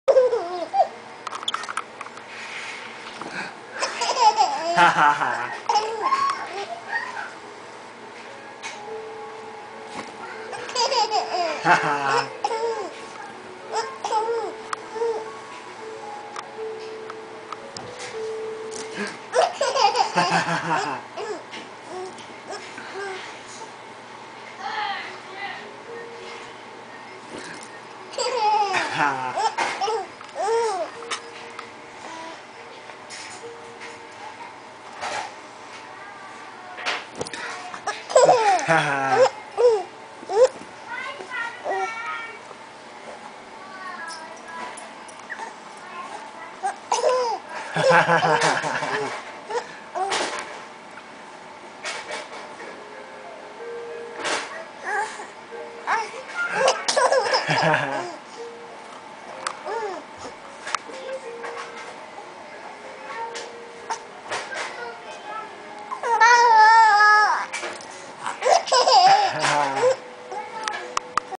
Lyndyn laughing...he loves playing peek a boo with daddy